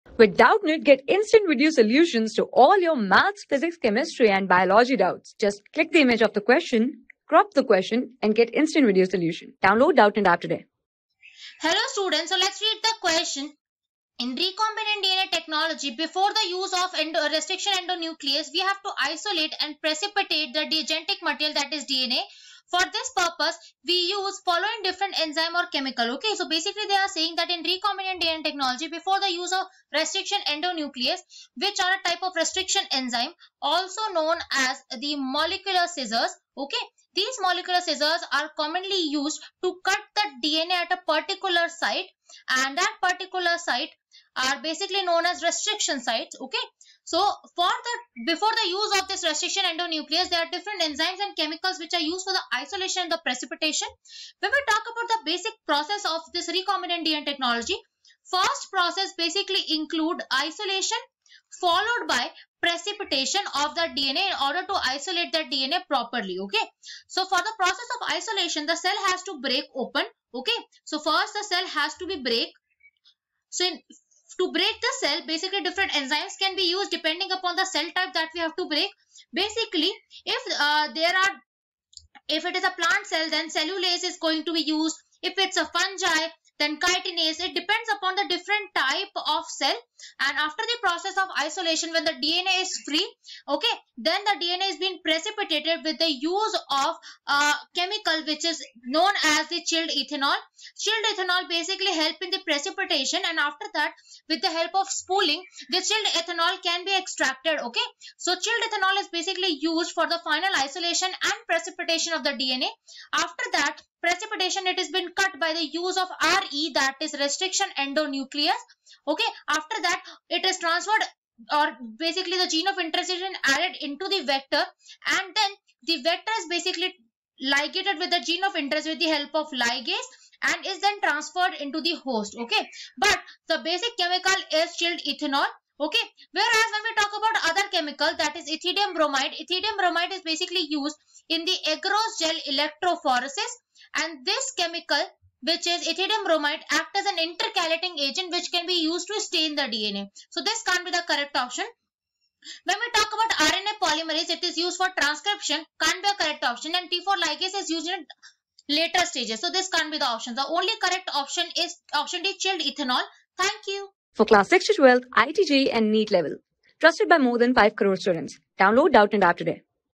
In recombinant DNA technology before the use of restriction endonuclease, we have to isolated an...
In recombinant DNA technology before the use of restriction endonuclease, we have to isolated and precipitate thegenetic material (DNA) . For this purpose we use following different enzyme / chemical :(a) Ethidium bromide \n\n(b) RNA polymerase \n\n(c) T4 ligase \n\n(d) Chilled Ethanol
Class: 12
Subject: BIOLOGY
Chapter: NTA NEET SET 68
Board:NEET

👉 Have Any Query? Ask Us.